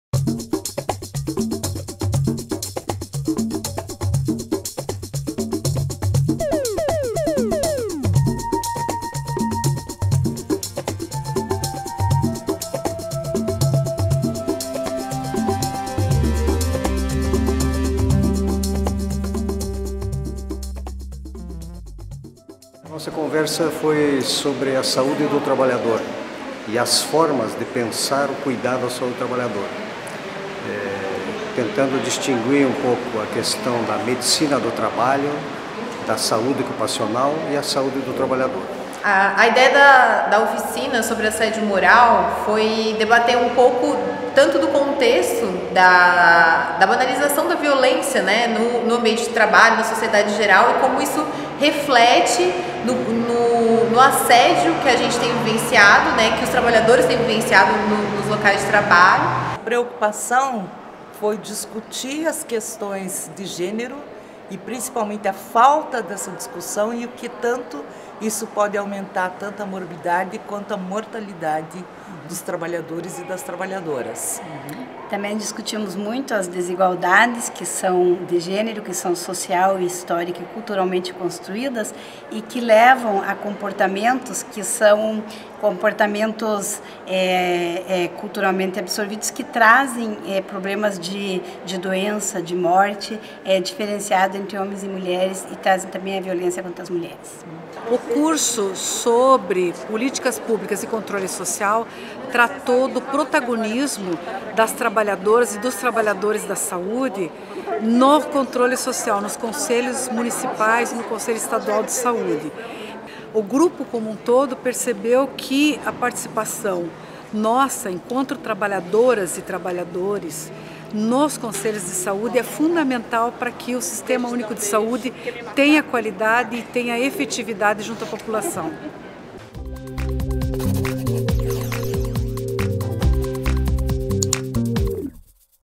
SINDTV I Confira mais sobre os minicursos do 8º Congresso do SindSaúde
Um dos momentos mais interessantes do 8º Congresso do SindSaúde, realizado no final de março no litoral do Estado, foram os minicursos. Diante da complexidade de assuntos que permeiam a nossa vida enquanto servidora e servidor militante, o Sindicato convidou oito especialistas em temas variados para dar palestras.

Delegadas e delegados puderam escolher entre temas como a reforma trabalhista, assédio moral, modelos de privatização, gênero, saúde da trabalhadora e do trabalhador, ação sindical, políticas públicas e Controle Social e ainda ParanaPrevidência e Contrarreforma da Previdência.

No vídeo a seguir, produzido pela SindTV, você confere um pouco do que rolou nesse importante momento da categoria.